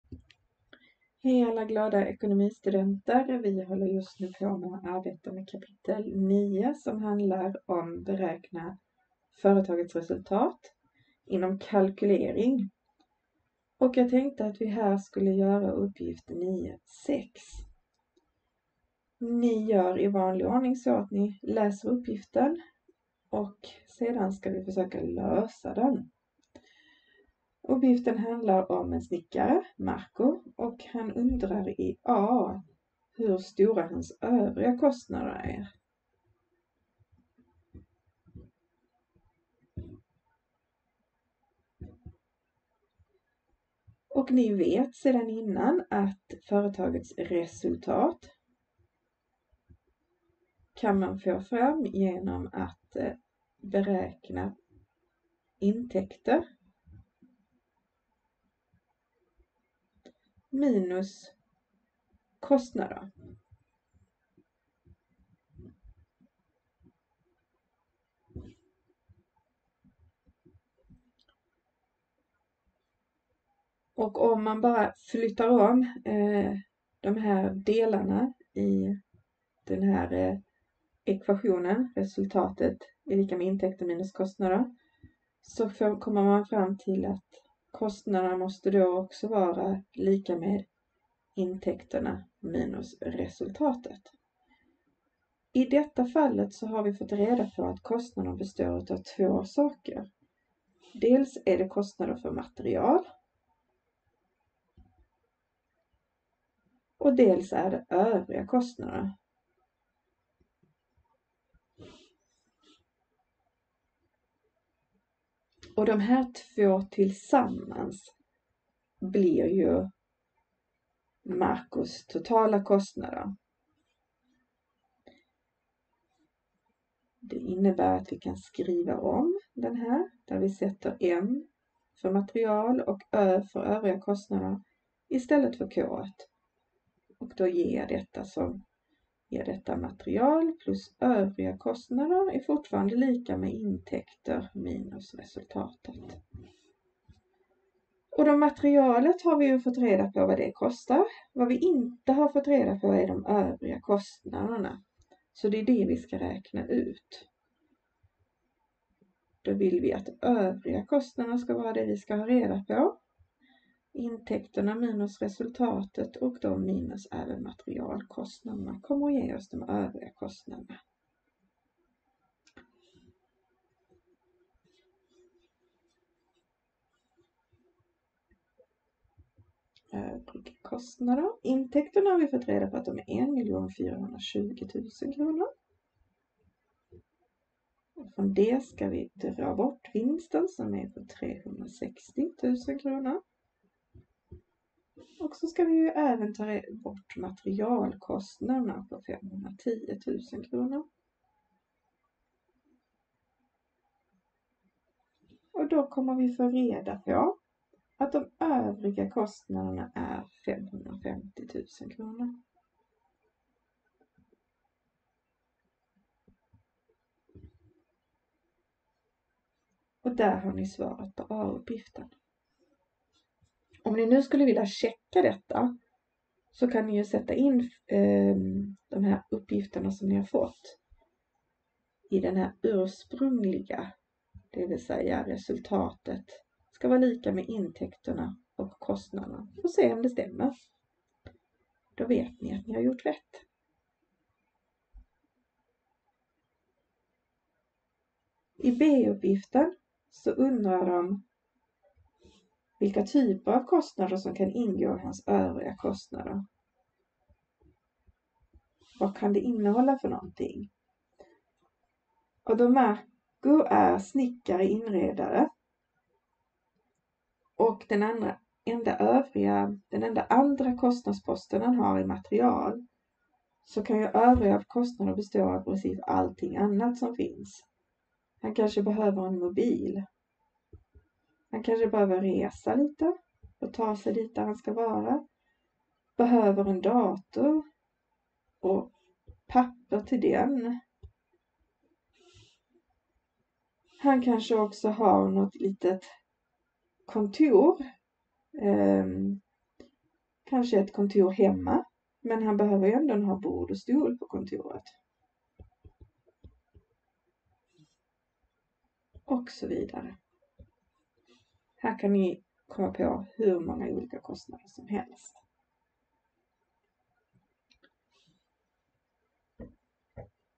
Resultat, intäkter och kostnader 9:6 - Företagsekonomi 1🍀🌸
Hur hänger dessa ihop? Genom att intäkter - kostnader = resultat. Ämnet är företagsekonomi och i denna video gör vi en uppgift på detta. / Maria Vinblad

Bok: Företagsekonomi 1 E3000 (E2000)